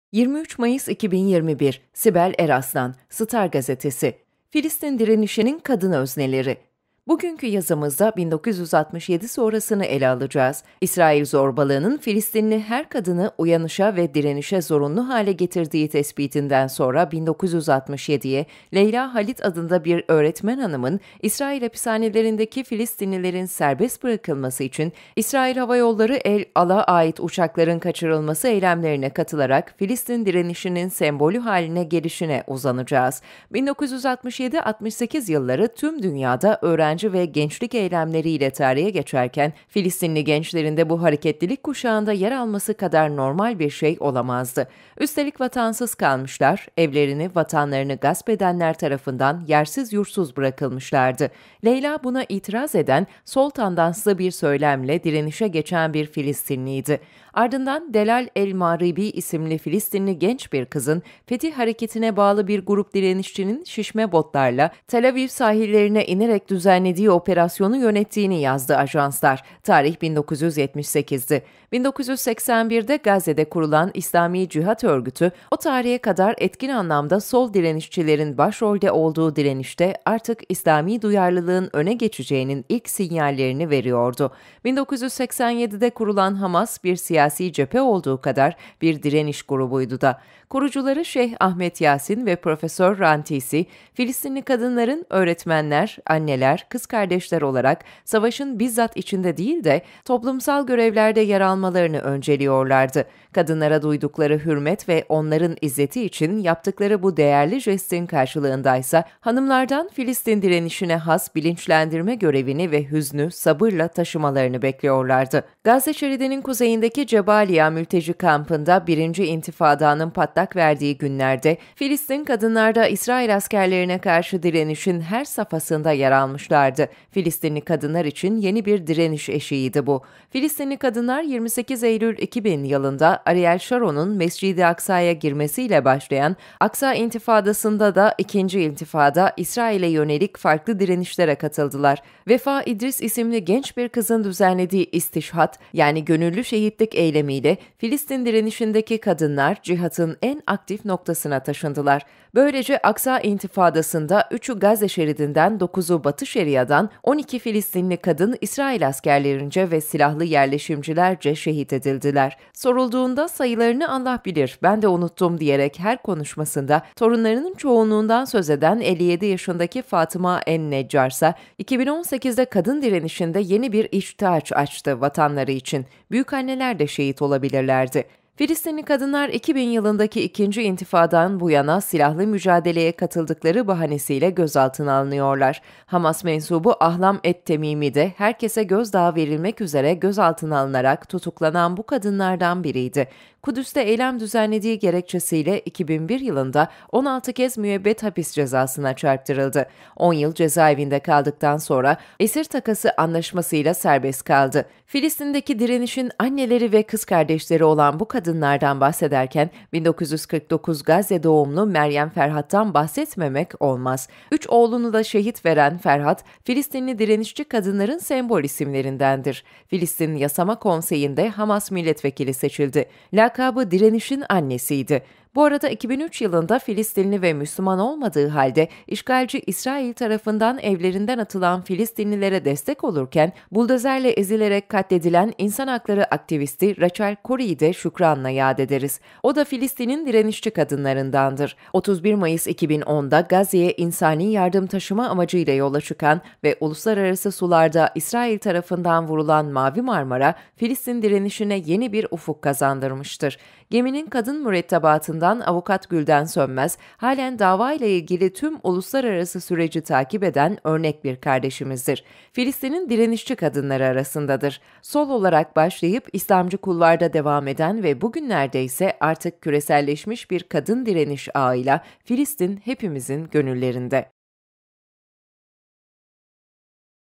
Filistin direnişinin kadın özneleri (2) - Sibel Eraslan
Bugünkü yazımızda 1967 sonrasını ele alacağız. İsrail zorbalığının Filistinli her kadını uyanışa ve direnişe zorunlu hale getirdiği tespitinden sonra 1967'ye, Leyla Halid adında bir öğretmen hanımın, İsrail hapishanelerindeki Filistinlilerin serbest bırakılması için İsrail Havayolları El Al'a ait uçakların kaçırılması eylemlerine katılarak Filistin direnişinin sembolü haline gelişine uzanacağız. 1967-68 yılları tüm dünyada öğrenci ve gençlik eylemleriyle tarihe geçerken, Filistinli gençlerin de bu hareketlilik kuşağında yer alması kadar normal bir şey olamazdı. Üstelik, vatansız kalmışlar, evlerini, vatanlarını gasp edenler tarafından yersiz yurtsuz bırakılmışlardı. Leyla, buna itiraz eden, sol tandanslı bir söylemle direnişe geçen bir Filistinliydi...

Ardından Delal el-Mağribi isimli Filistinli genç bir kızın, Fetih Hareketi'ne bağlı bir grup direnişçinin şişme botlarla Tel Aviv sahillerine inerek düzenlediği operasyonu yönettiğini yazdı ajanslar, tarih 1978'di.

1981'de Gazze'de kurulan İslami Cihat Örgütü o tarihe kadar etkin anlamda sol direnişçilerin başrolde olduğu direnişte artık İslami duyarlılığın öne geçeceğinin ilk sinyallerini veriyordu. 1987'de kurulan Hamas, bir siyasi cephe olduğu kadar bir direniş grubuydu da. Kurucuları Şeyh Ahmet Yasin ve Prof. Rantisi, Filistinli kadınların, öğretmenler, anneler, kız kardeşler olarak savaşın bizzat içinde değil de, toplumsal görevlerde yer almalarını önceliyorlardı. Kadınlara duydukları hürmet ve onların izzeti için yaptıkları bu değerli jestin karşılığındaysa hanımlardan Filistin direnişine has bilinçlendirme görevini ve hüznü sabırla taşımalarını bekliyorlardı.

Gazze Şeridi'nin kuzeyindeki Cebaliya Mülteci Kampı'nda 1'inci İntifada'nın patlak verdiği günlerde (1987), Filistinli kadınlar da İsrail askerlerine karşı direnişin her safhasında yer almışlardı. Filistinli kadınlar için yeni bir direniş eşiğiydi bu...

Filistinli kadınlar, 28 Eylül 2000 yılında Ariel Şaron'un Mescid-i Aksa'ya girmesiyle başlayan Aksa İntifadası'nda da (2. İntifada) İsrail'e yönelik farklı direnişlere katıldılar. Vefa İdris isimli genç bir kızın düzenledğii istişhad, yani gönüllü şehitlik eylemiyle, Filistin direnişindeki kadınlar, cihadın en aktif noktasına taşındılar. Böylece Aksa İntifadası'nda 3'ü Gazze Şeridi'nden, 9'u Batı Şeria'dan, 12 Filistinli kadın, İsrail askerlerince ve silahlı yerleşimcilerce şehit edildiler.

Sorulduğunda 'sayılarını Allah bilir, ben de unuttum' diyerek her konuşmasında torunlarının çokluğundan söz eden 57 yaşındaki Fatıma en-Neccar ise, 2018'de kadın direnişinde yeni bir içtihat açtı; vatanları için, 'büyükanneler de şehit olabilirler'di.

Filistinli kadınlar, 2000 yılındaki İkinci İntifada'dan bu yana "silahlı mücadeleye" katıldıkları bahanesiyle gözaltına alınıyorlar... Hamas mensubu Ahlam et-Temimi de herkese gözdağı verilmek üzere gözaltına alınarak tutuklanan bu kadınlardan biriydi. Kudüs'te eylem düzenlediği gerekçesiyle, 2001 yılında 16 kez müebbet hapis cezasına çarptırıldı. 10 yıl cezaevinde kaldıktan sonra Hamas ile İsrail arasında yapılan esir takası anlaşmasıyla serbest kaldı.

Filistin'deki direnişin anneleri ve kız kardeşleri olan bu kadınlardan bahsederken, 1949 Gazze doğumlu Meryem Ferhat'tan bahsetmemek olmaz. Üç oğlunu da şehit veren Ferhat, Filistinli direnişçi kadınların sembol isimlerindendir. Filistin Yasama Konseyi'nde Hamas milletvekili seçildi. Lakabı: "Direnişin annesi"ydi.

Bu arada 2003 yılında Filistinli ve Müslüman olmadığı halde, işgalci İsrail tarafından evlerinden atılan Filistinlilere destek olurken, buldozerle ezilerek katledilen insan hakları aktivisti Rachel Corie'yi de şükranla yad ederiz. O da Filistin'in direnişçi kadınlarındandır.

31 Mayıs 2010'da Gazze'ye insani yardım taşıma amacıyla yola çıkan ve uluslar arası sularda İsrail tarafından vurulan Mavi Marmara, Filistin direnişine yeni bir ufuk kazandırmıştır. Geminin kadın mürettebatından avukat Gülden Sönmez, halen davayla ilgili tüm uluslar arası süreci takip eden örnek bir kardeşimizdir, Filistin'in direnişçi kadınları arasındadır.

Sol olarak başlayıp, İslamcı kulvarda devam eden ve bugünlerdeyse artık küreselleşmiş bir kadın direniş ağıyla, Filistin hepimizin gönüllerinde...